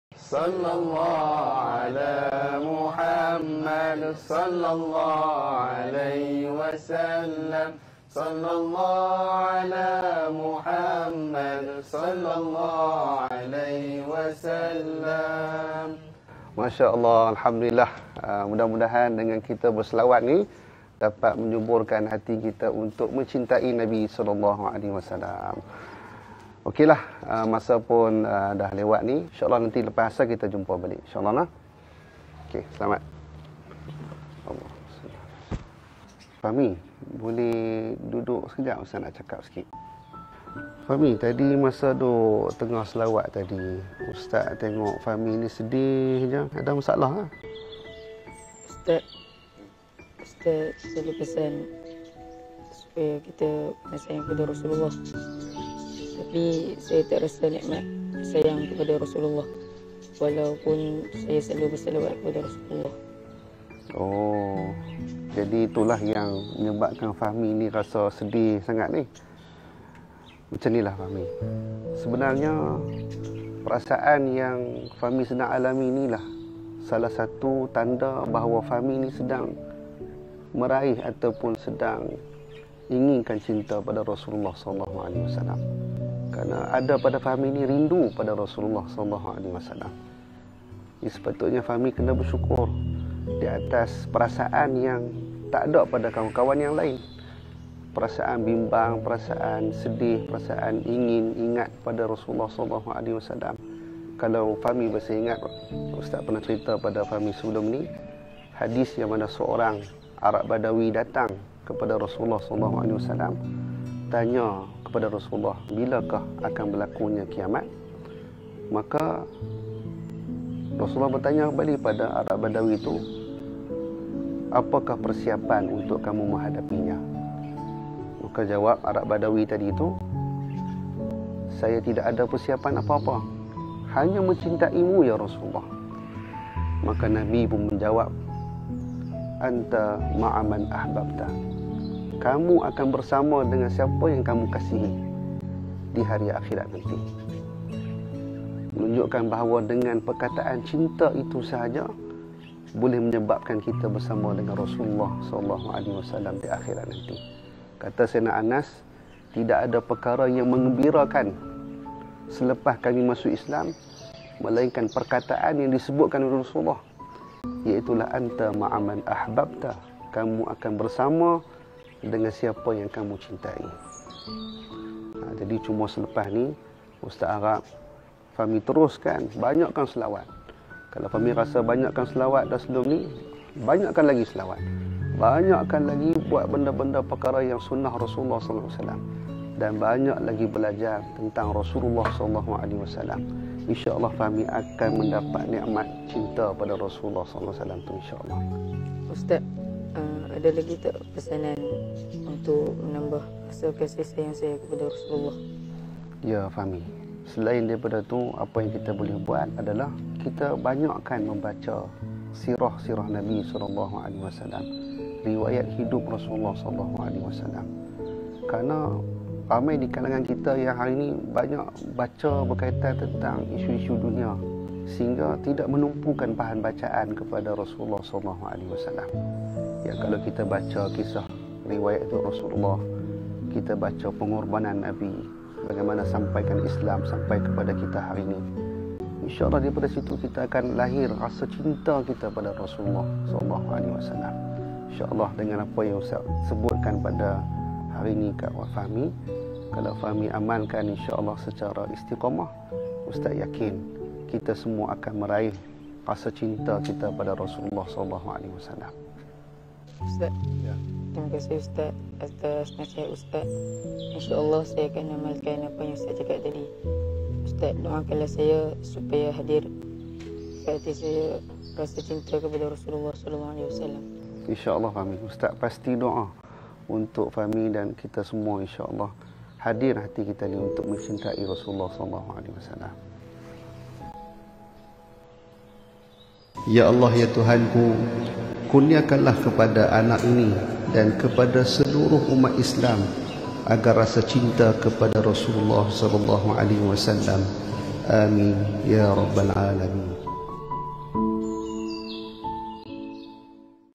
Apa itu NIKMAT CINTA yang sebenarnya? Cinta yang bahagia membawa ke syurga.
Susah untuk wujudkan rasa CINTA di hati ini. Rasa resah hadir memikirkan hati ini yang belum diketuk oleh sapaan CINTA. Hadirlah CINTA untuk aku nikmati.

Allahumasollialasaidinamuhammad...